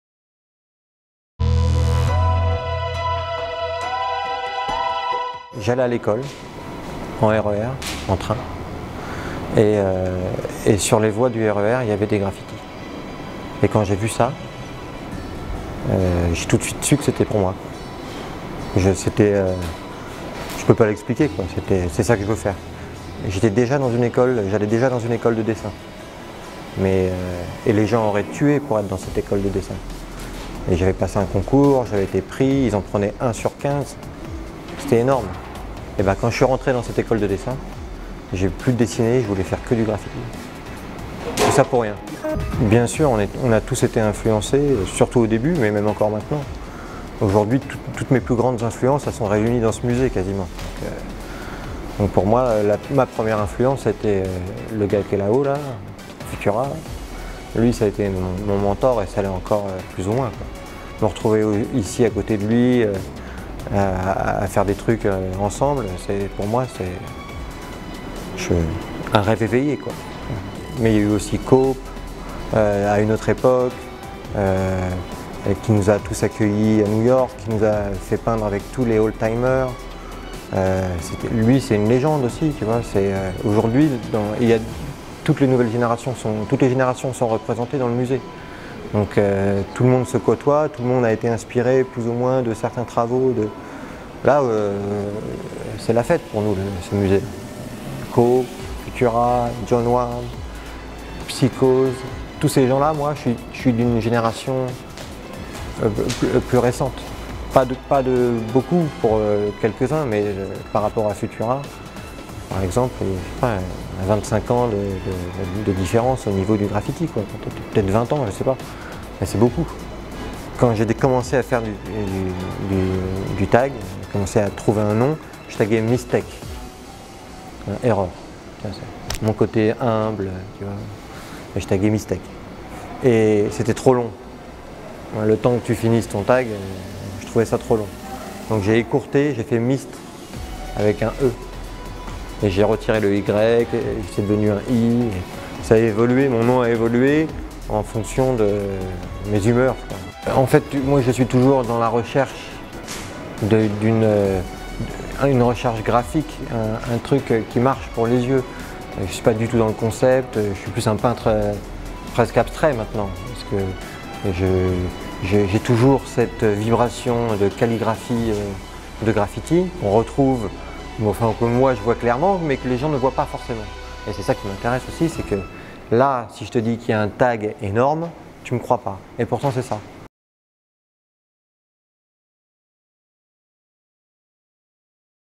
Mist Premier Artiste Street Art Français
Mist est né à Paris en 1972, et découvre le graffiti en 1988 sur le chemin de l’école, où il étudie le design durant 4 ans. Il a ensuite appris la sculpture dans une société de figurines durant une année. En 1992, il rejoint le groupe d’artistes graffitis les COP (Control of Paris) et peint de nombreux murs. Bien que fasciné par le lettrage Wild Style, il a développé son propre style calligraphique.